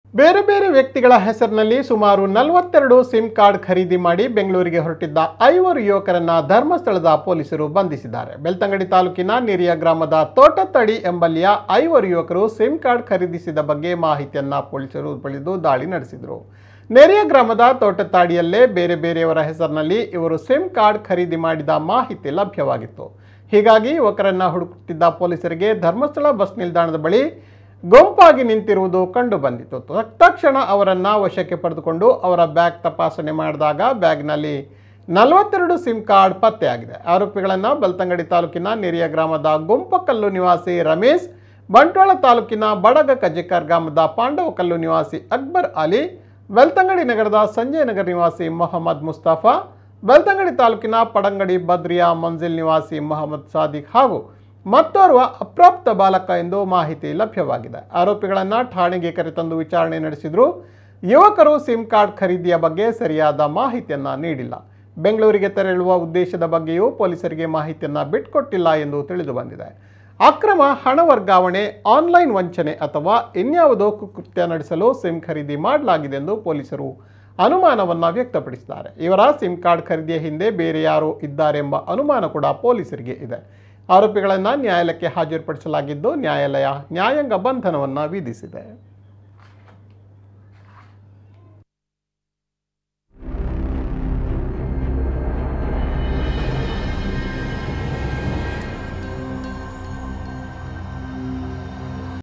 42 ಸಿಮ್ ಕಾರ್ಡ್ ಖರೀದಿ ಮಾಡಿ ಬೆಂಗಳೂರಿಗೆ ಹೊರಟಿದ್ದ ಐವರು ಯುವಕರು ಅರೆಸ್ಟ್..! ಆನ್ಲೈನ್ ವಂಚನೆ, ಕುಕೃತ್ಯ ಅನುಮಾನ.!
NAMMA KUDLA news 24x7
ತಾಜಾ ಸುದ್ದಿಗಳಿಗೆ  ನಮ್ಮ ಕುಡ್ಲ whatapp ಗ್ರೂಪ್‌ಗೆ  ಸೇರಿ..